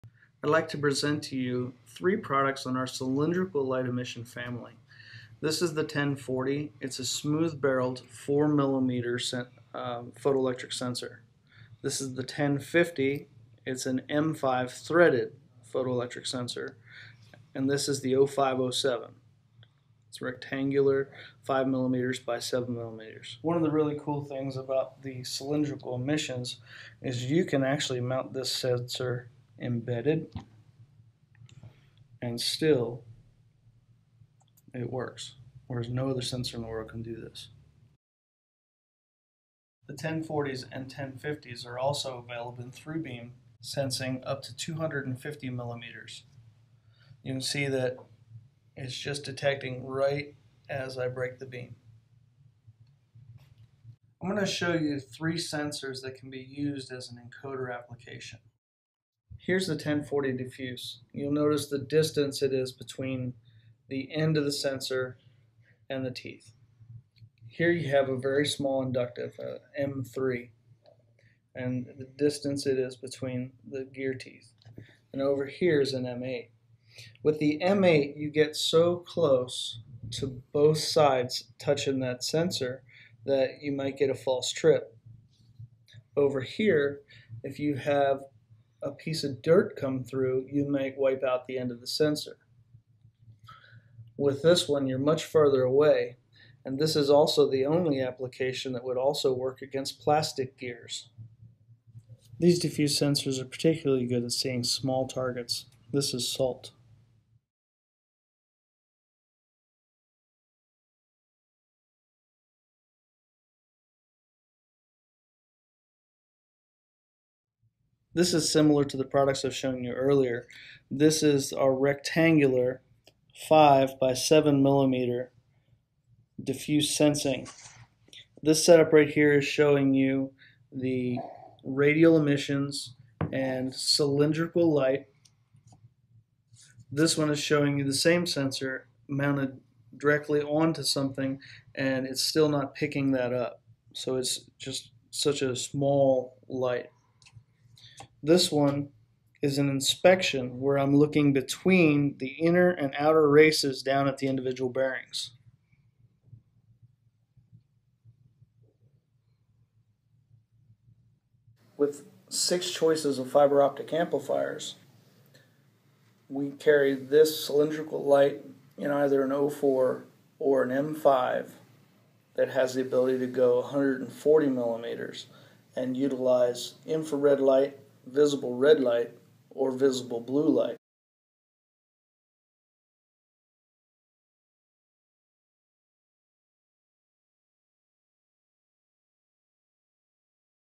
Contrinex Sub-Miniature Photoelectric (demonstration)
Demonstration on using the Contrinex 1040, 1050 and 0507 for a wide range of industrial sensing applications. Size range of 4 mm to 7mm.